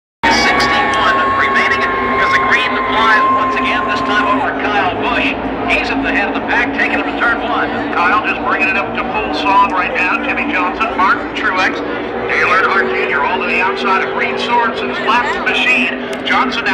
Talladega Race fan falls on face: "...don't let him up!"
The Chicken Men watch an inebriated fan fall on his face while attempting to walk over chairs on his way to the restroom.
( signs posted everywhere clearly state: " DO NOT STAND IN SEATS " ).

Security and medical staff were flagged over to his aide, along with many "paramedic wanna-bees" in the surrounding seats who would NOT leave him alone even after repeated statements of; "..I am FINE!" and "PLEASE LEAVE ME ALONE!! " haha. ( we will have none of that crazy talk sir! )

Even Chicken Man "Frank" was distracted from watching the race, but only for a flap or two ( oops, I meant " lap" or two ). Chicken Humor.